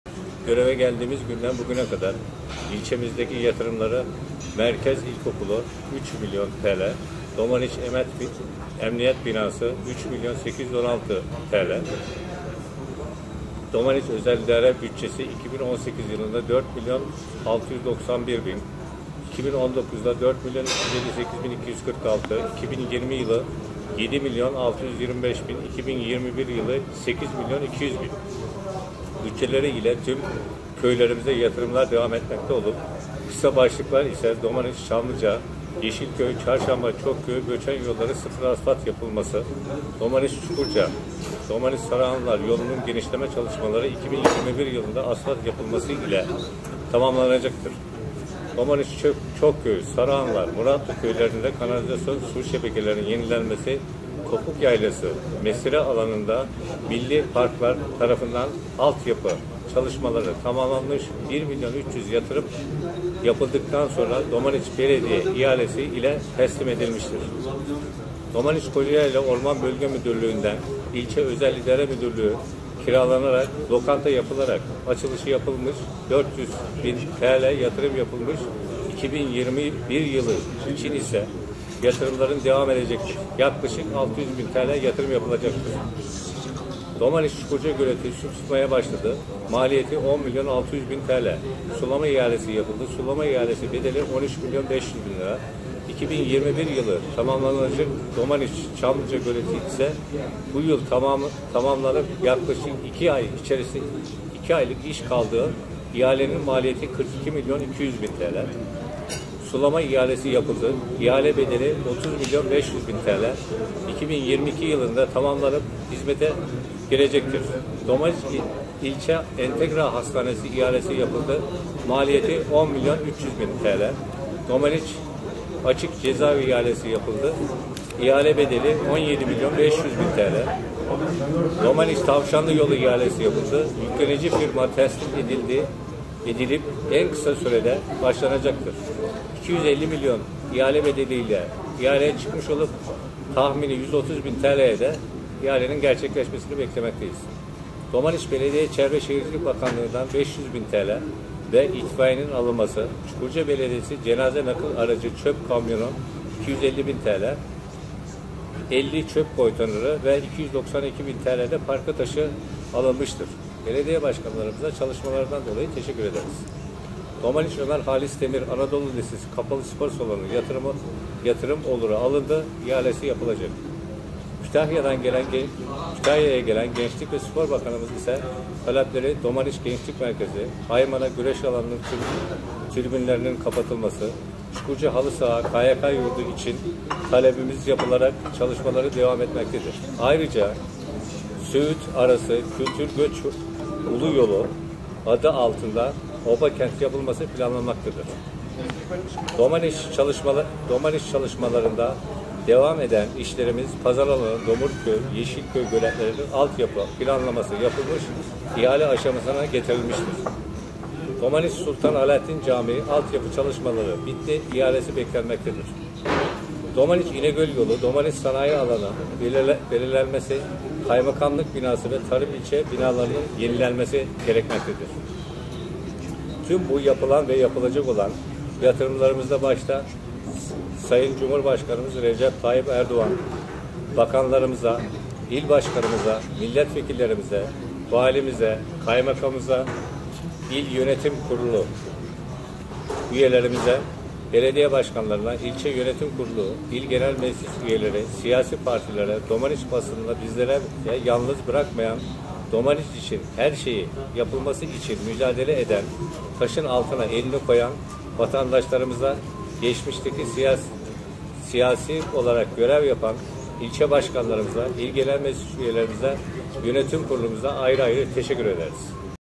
Domaniç’e yapılan ve yapılacak yatırımları anlattı
Ak Parti İlçe Başkanı Ahmet Özoğul gazetemize basın açıklamasında bulundu.Domaniç Gazetesi